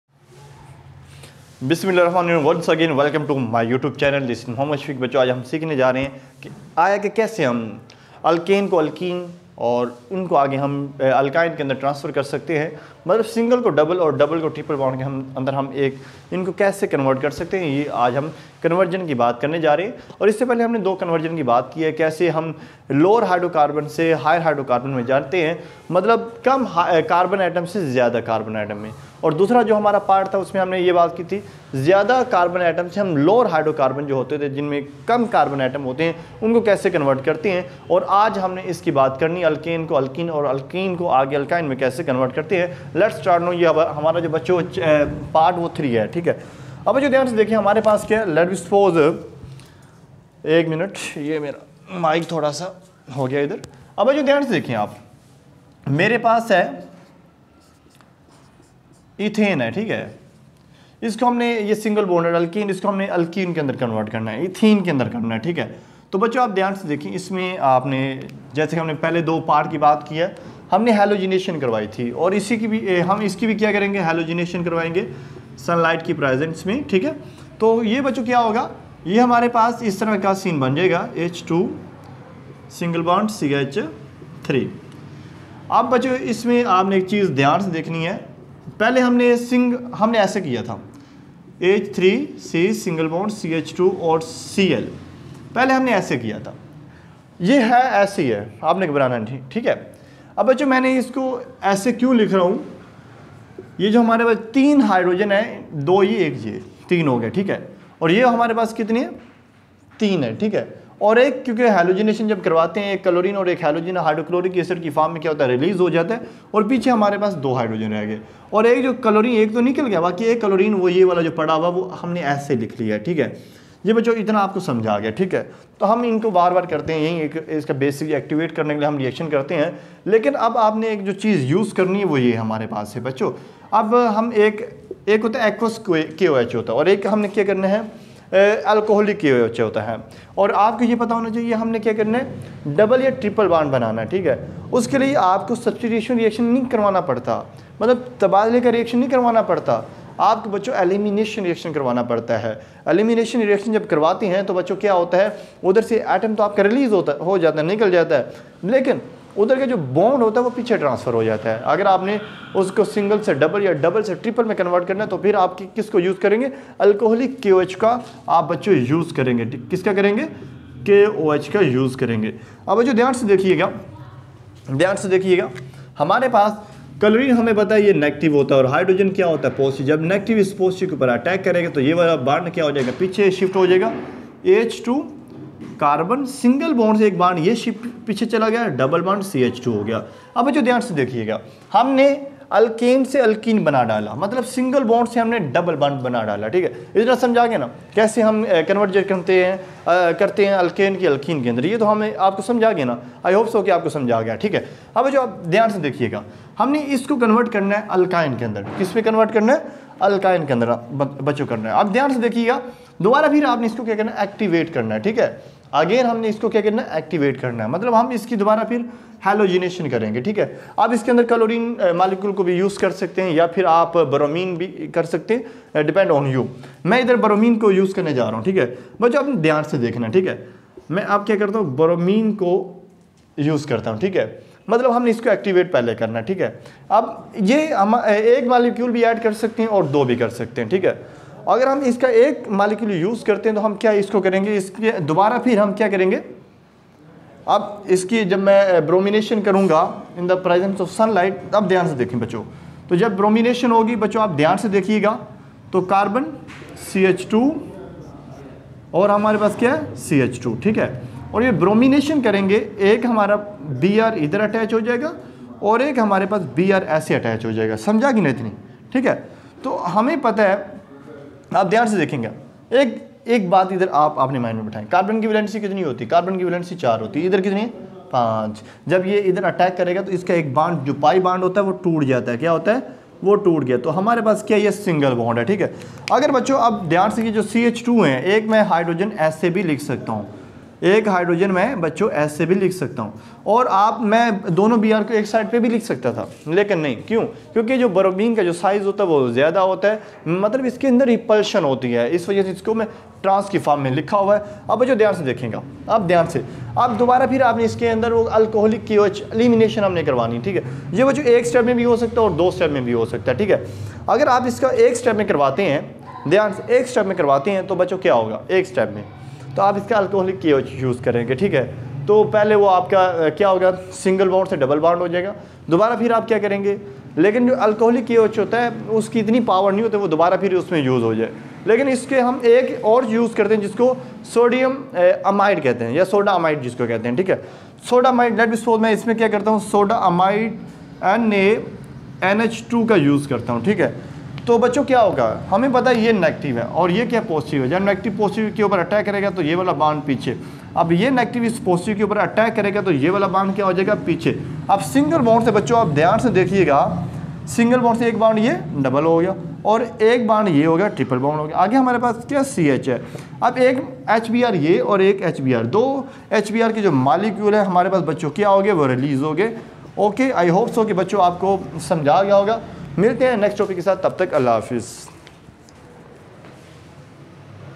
organic conversion class 12 || part 3|| how to convert alkane into alkene and alkyne
organic conversion class 12.conversion of alkane into alkene and alkyne.